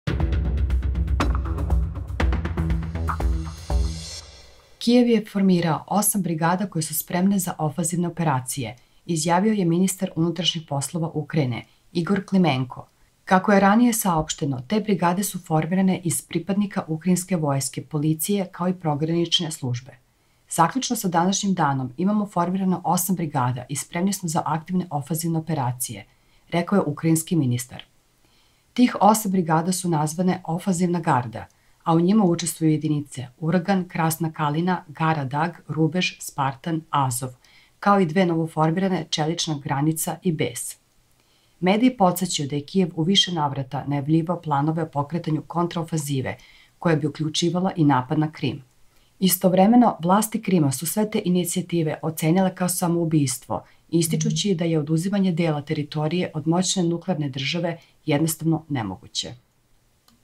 Kijev Formirao Osam Brigada Za Ofanzivne Akcije ! Rusija I Ukrajina Rat najnovije Vesti
Kijev je formirao osam brigada koje su spremne za ofanzivne operacije, izjavio je ministar unutrašnjih poslova Ukrajine, Igor Klimenko.
Kako je ranije saopšteno, te brigade su formirane iz pripadnike ukrajinske vojske, policije, kao i pogranične službe.
- Zaključno sa današnjim danom imamo formirano osam brigada i spremni smo za aktivne ofanzivne operacije - rekao je ukrajinski ministar.
Tih osam brigada su nazvane "Ofanzivna garda", a u njima učestvuju jedinice "Uragan", "Krasna kalina", "Kara-Dag", "Rubež", "Spartan", "Azov", kao i dve novoformirane – "Čelična granica" i "Bes".
Mediji podsećaju da je Kijev u više navrata najavljivao planove o pokretanju kontraofanzive koja bi uključivala i napad na Krim.
Istovremeno, vlasti Krima su sve te inicijative ocenile kao samoubistvo ističući da je "oduzimanje dela teritorije od moćne nuklearne države jednostavno nemoguće".